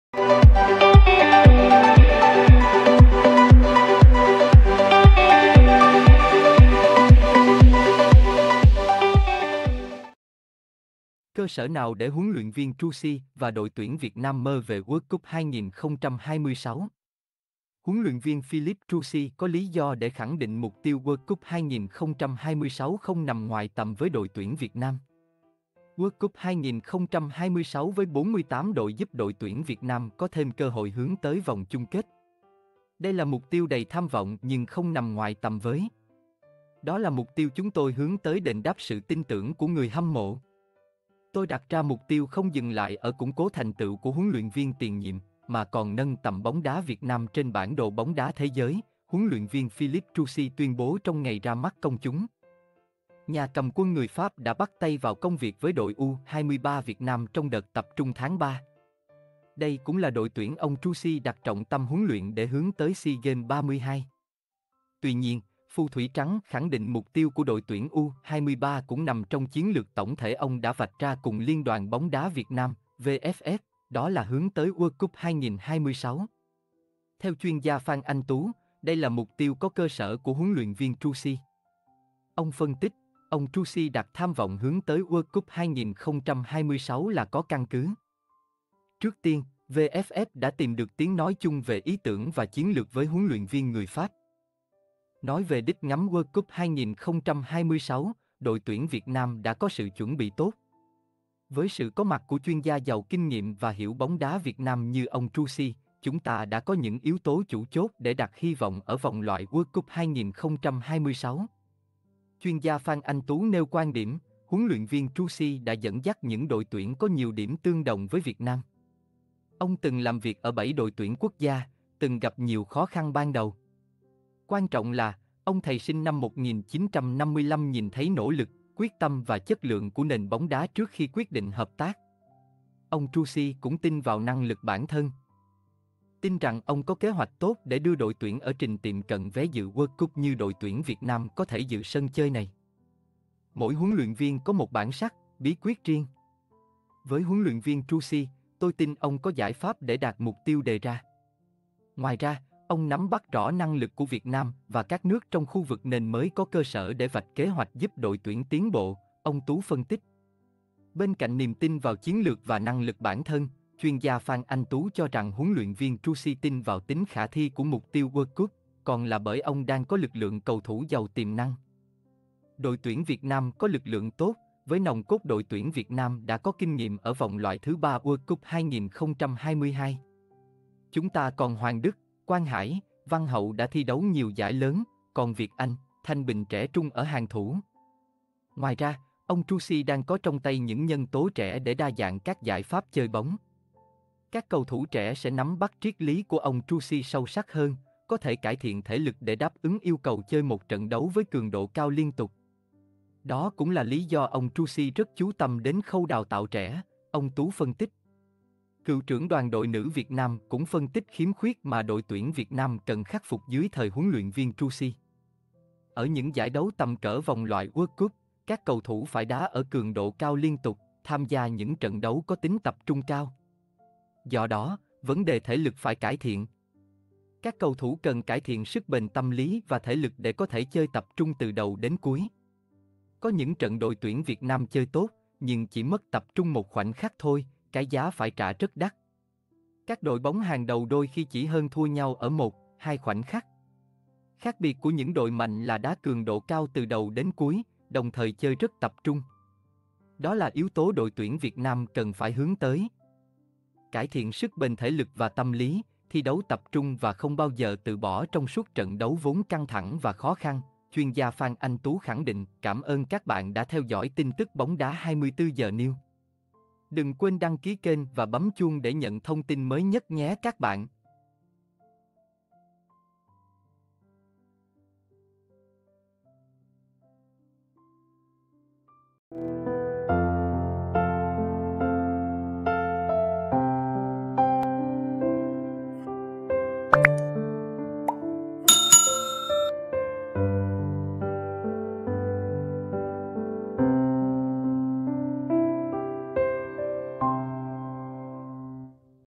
Cơ sở nào để HLV Troussier và đội tuyển Việt Nam mơ về World Cup 2026?
HLV Philippe Troussier có lý do để khẳng định mục tiêu World Cup 2026 không nằm ngoài tầm với đội tuyển Việt Nam.

00:00 Giới thiệu
00:11 Nội Dung